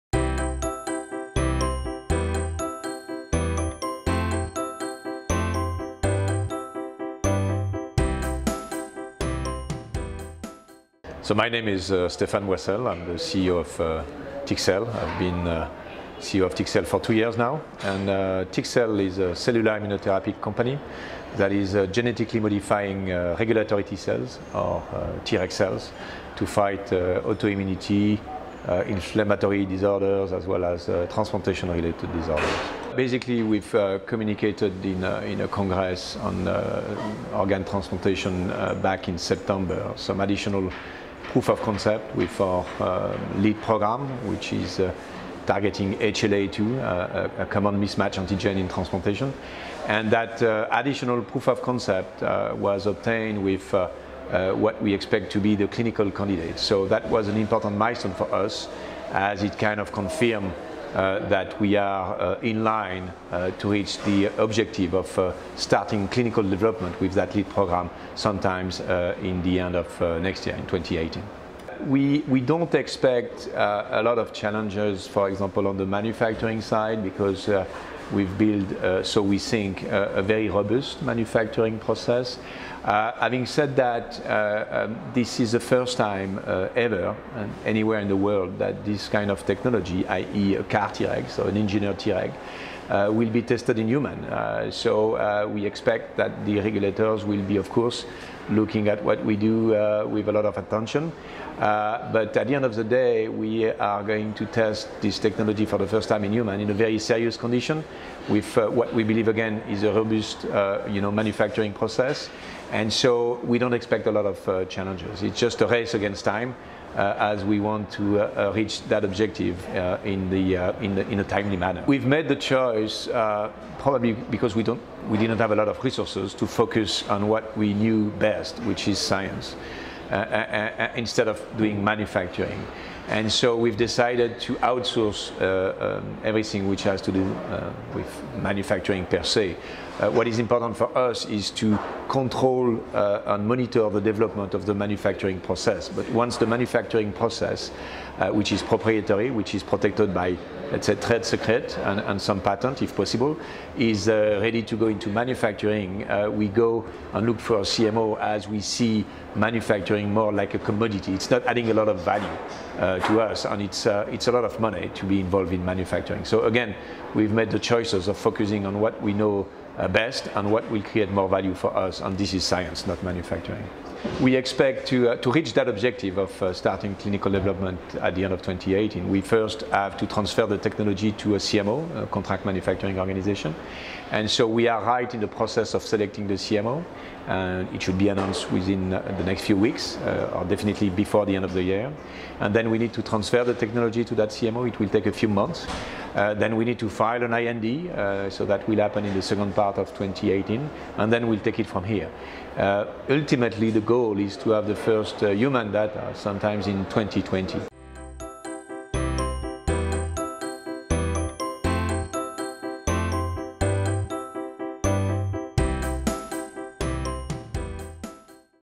Next generation CAR-Treg therapies: an interview with Stéphane Boissel
In this interview, filmed exclusively at the Alliance for Regenerative Medicine's 5th Annual European Union Investor Day, Stéphane Boissel, CEO, TxCell, discusses recent data on TxCell's proprietary CAR-Treg cell therapies. Speaking to RegMedNet, Stéphane also discusses TxCell's preparations for clinical trial and commercial manufacture.